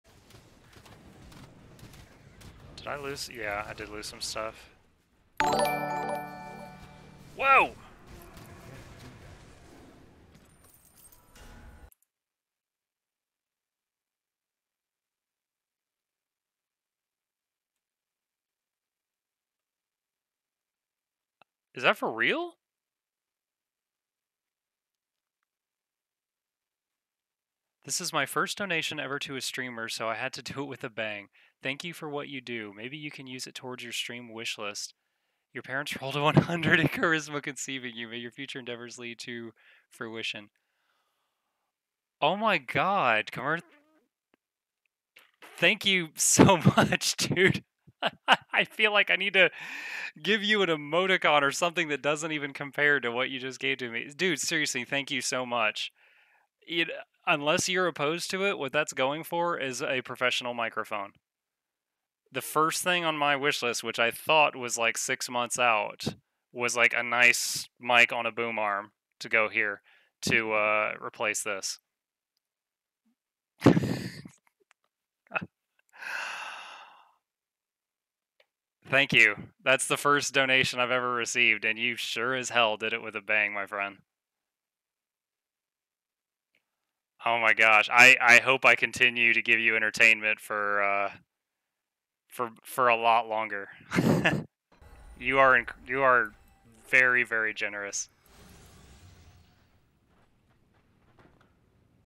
$100 donation breaks Mukluk
Khmergodpc sent me my first ever donation today and broke me for about 10 seconds while I stared slackjawed at the message. I was sure I was misreading "bits" for "$" and re-read it about 5 times.